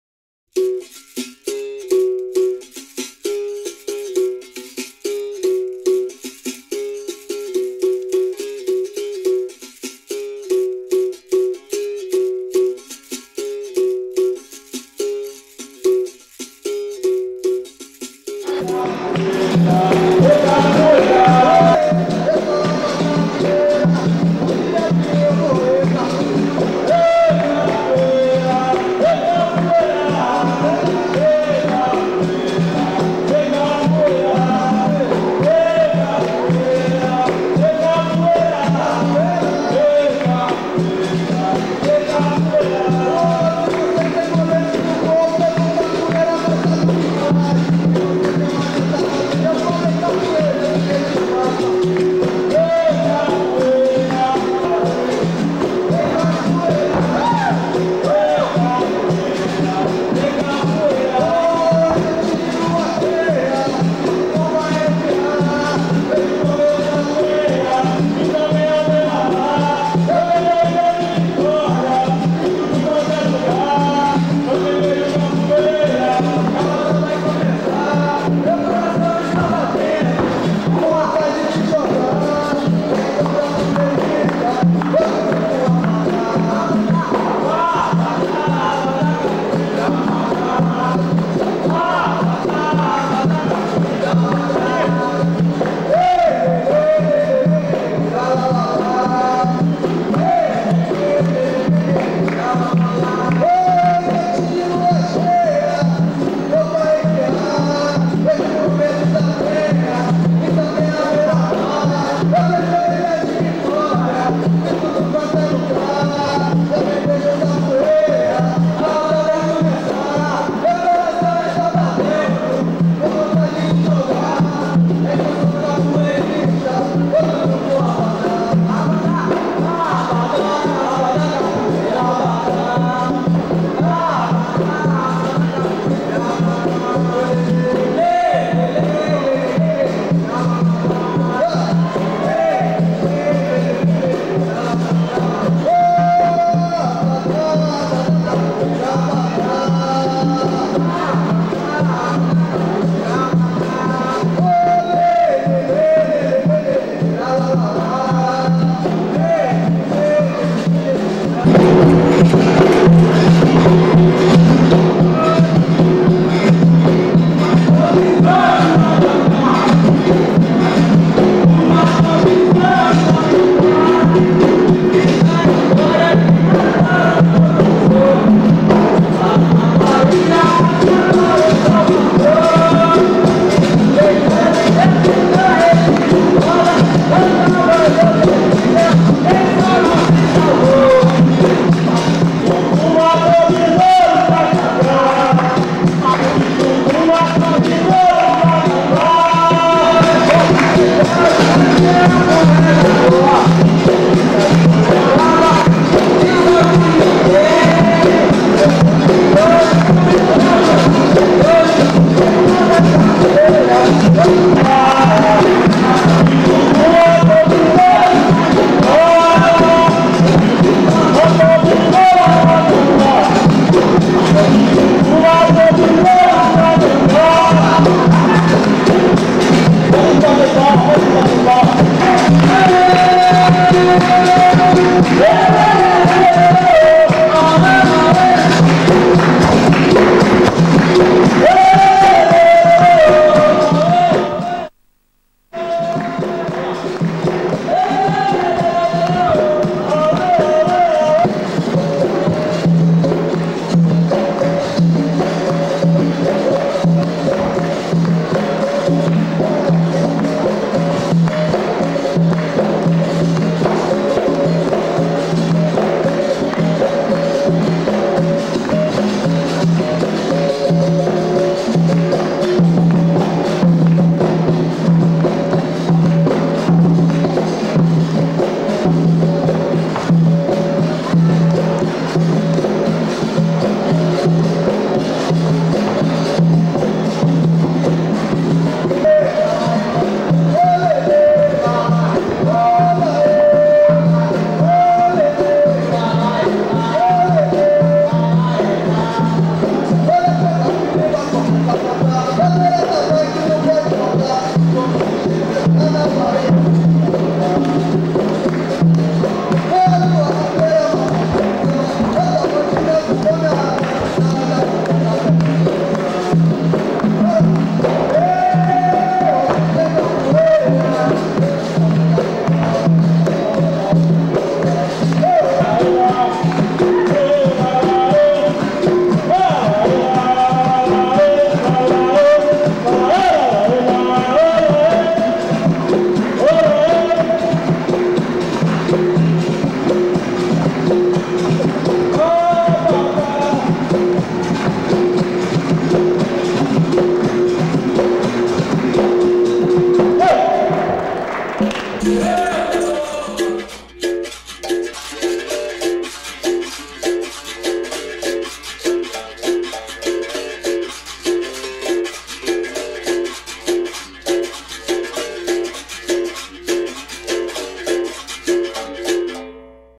Mestre IramCustodio etsesélèvesAbada-Capoeira démonstrationcapoeira LePlessisRobinson27-10-1997-6m20
Mestre Iram Custodio et ses élèves Abada-Capoeira démonstration de capoeira au Forum des Associations Le Plessis Robinson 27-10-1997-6m20